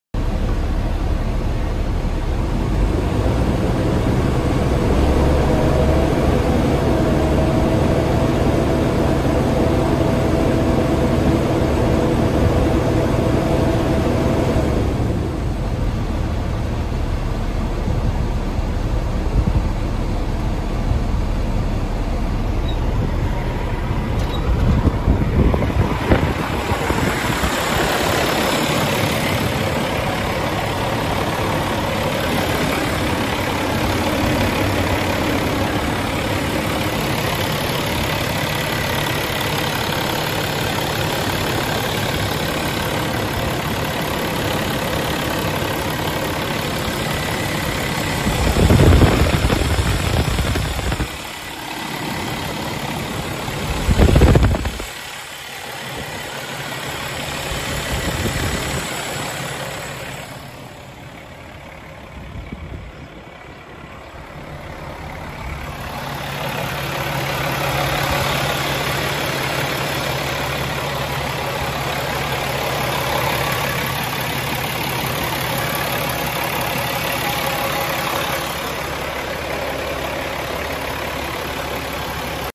2008 PETERBILT 384 For Sale
2008 Peterbilt 384 single axle, Caterpillar C13, 10 speed, 4.10 ratio, 157 inch wheel base, 12,900 lb stamp weight, aluminum fenders, clean low mile truck, state DOT inspected, delivery and financing available